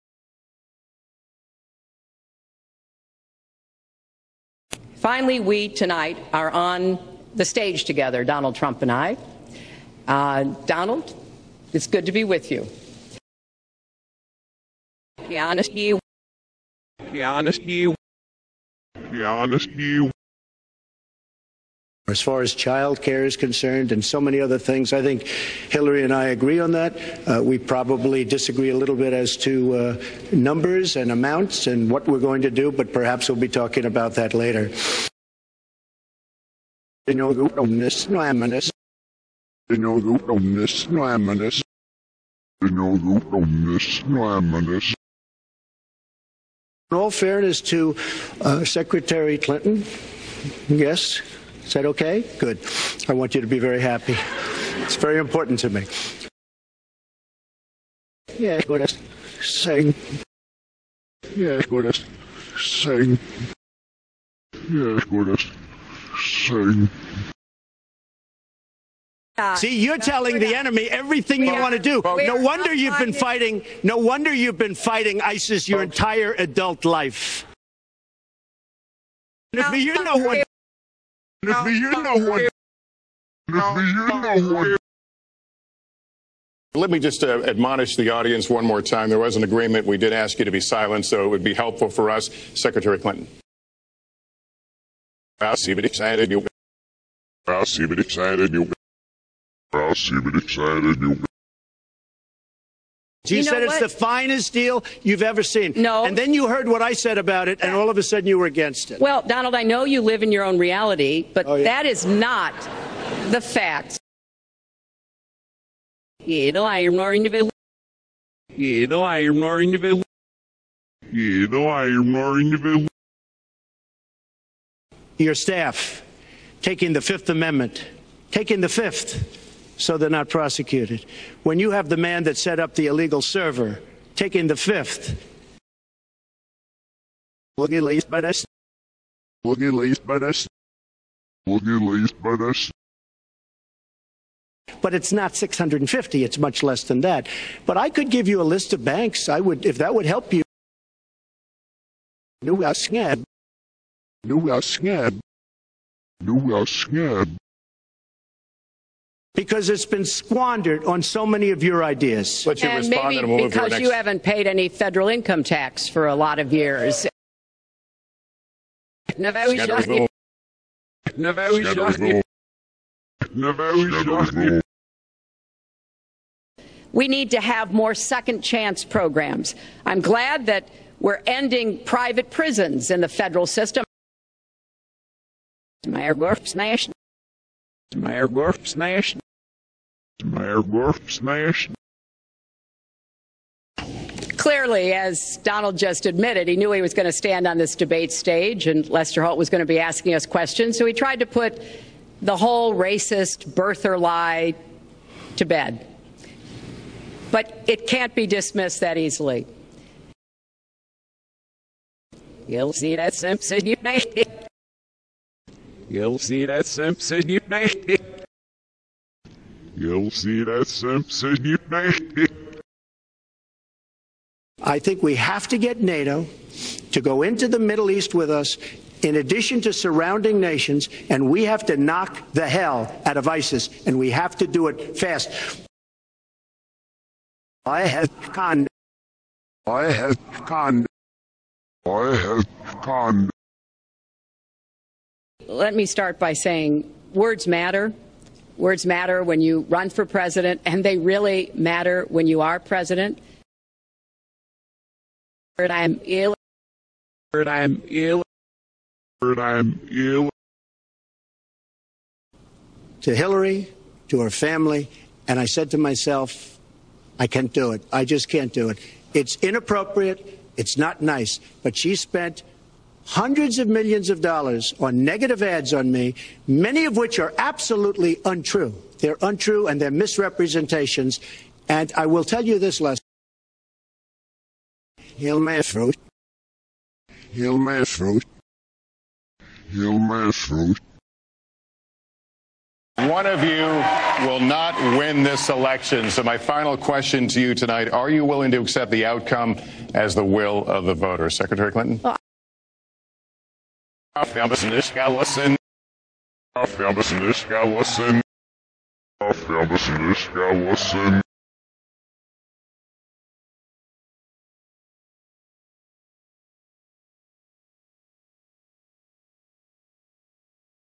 Reverse Speech analysis of the first Presidential debate - Sept 2016
Reverse Speech analysis of the first Presidential debate between Donald Trump and Hillary Clinton September 2016 - these are selected reversals only - the full transcript can be seen